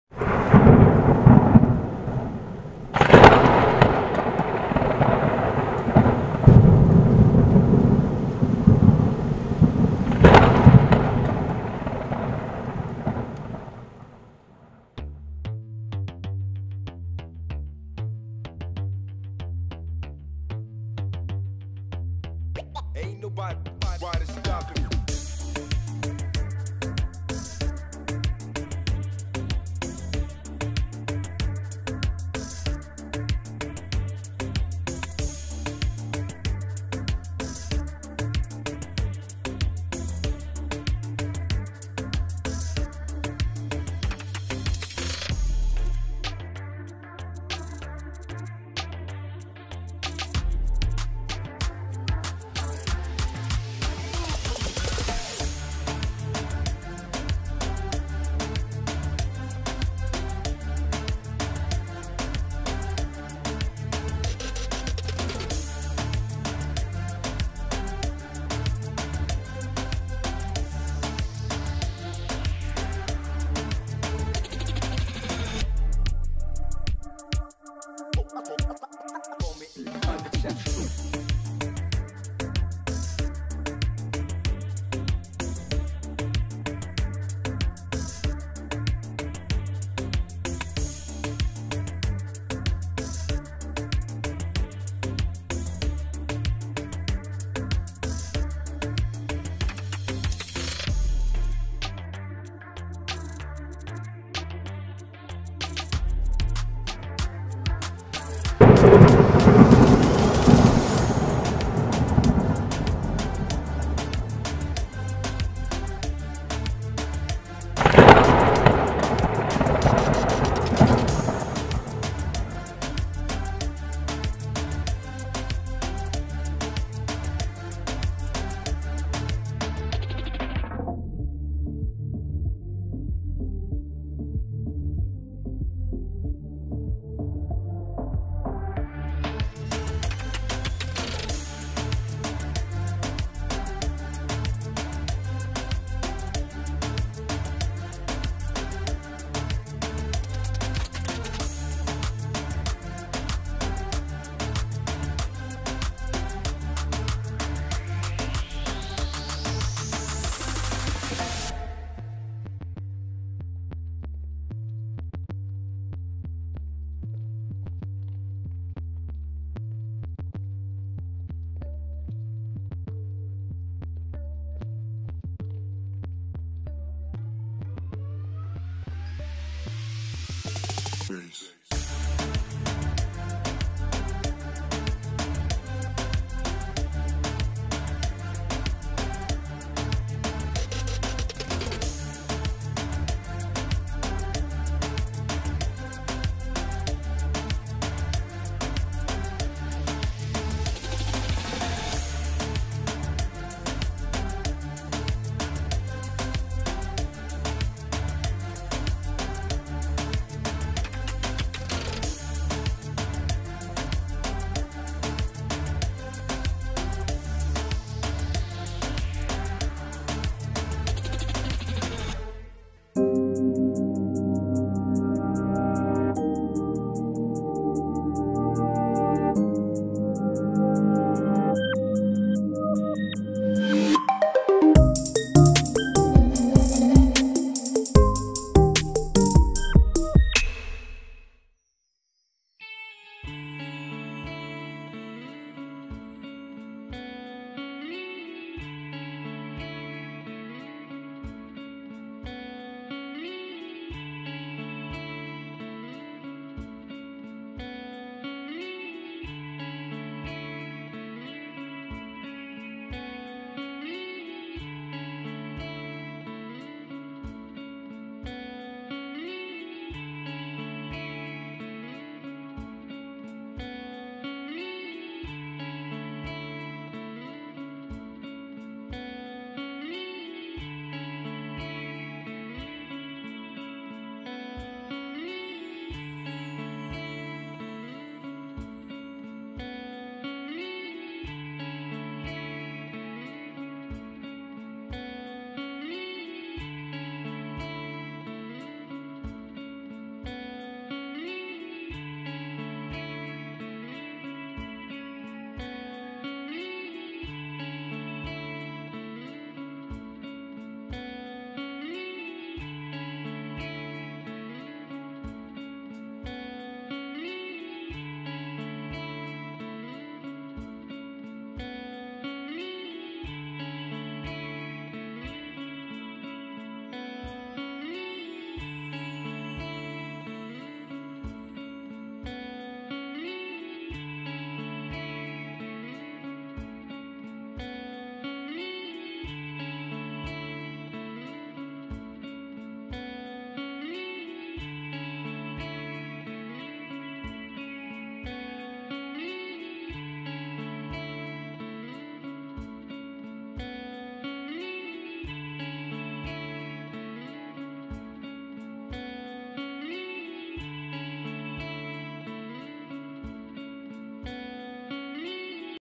DRONE MURAH BERKWALITAS TERBANG DI ATAS SUNGAI DESA NGRAPAH
PERTEMUAN 3 SUNGAI GALEH ,GUMUK,KLEGUNG DESA NGRAPAH